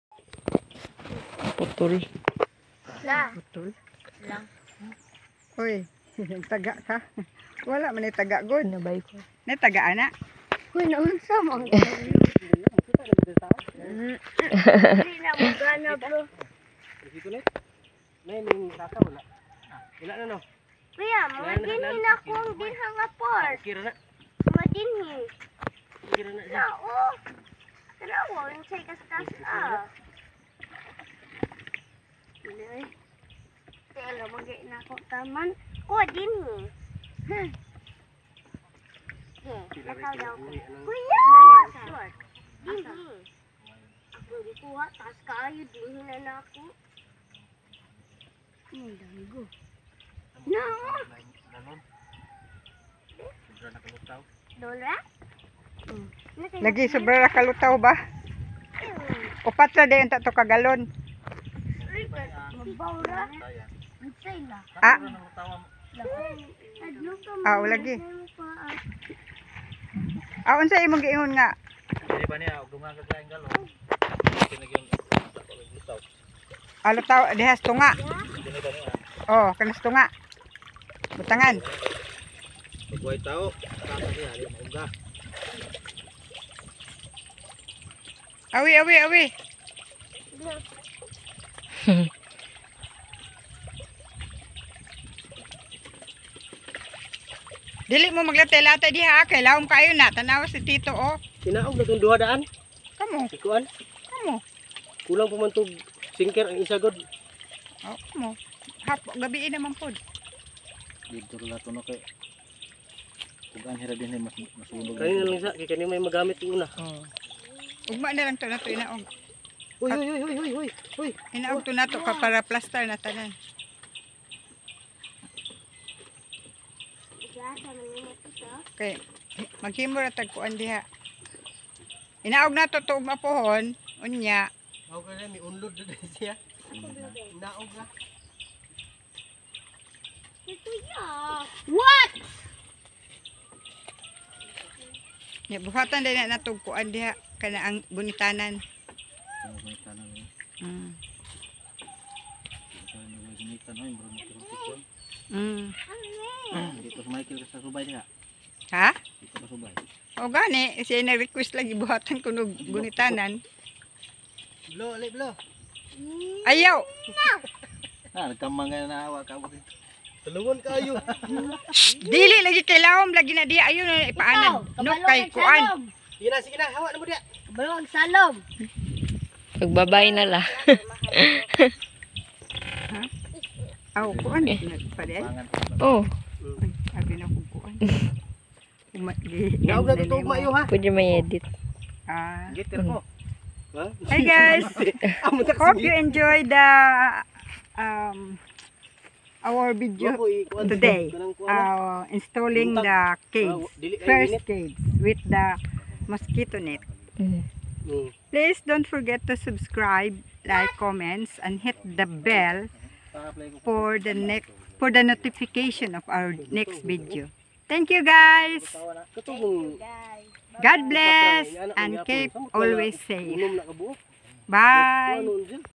INSTALLING THE 1ST FISH CAGE WITH THE MOSQUITO NET
Our first fish cage with mosquto net has been installed.